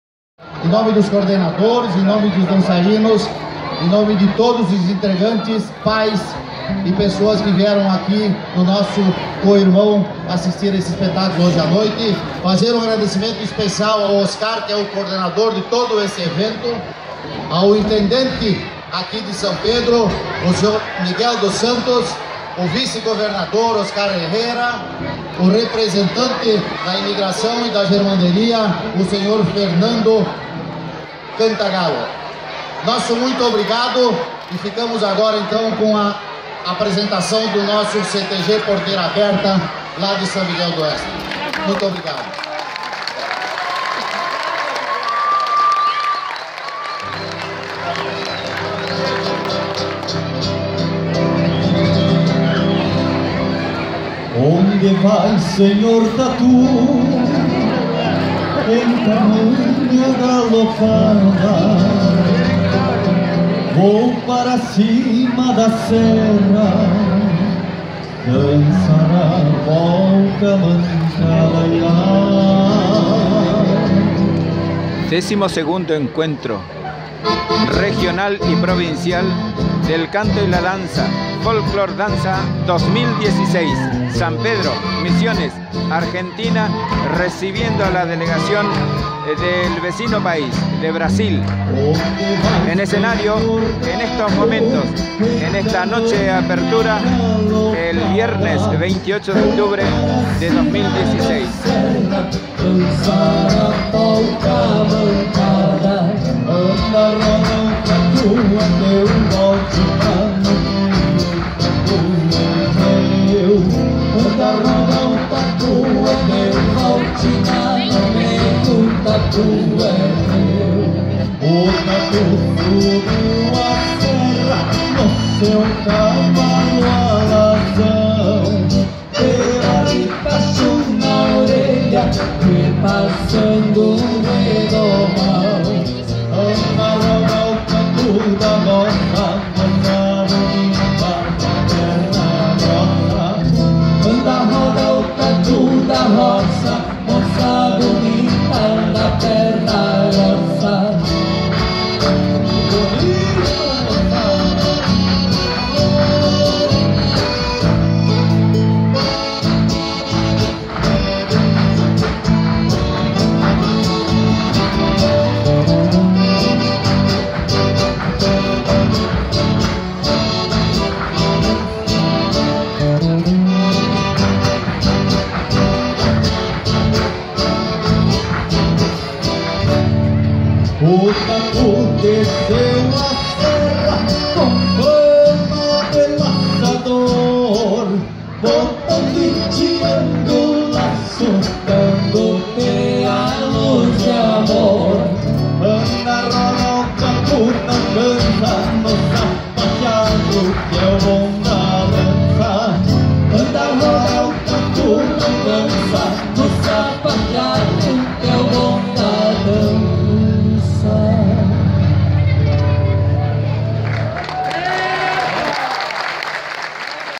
Folklodanza 2016 - 2
Segunda parte de "CTG de San Miguel Oeste Brasil en el XII Encuentro Provincial y Regional del Canto y la Danza.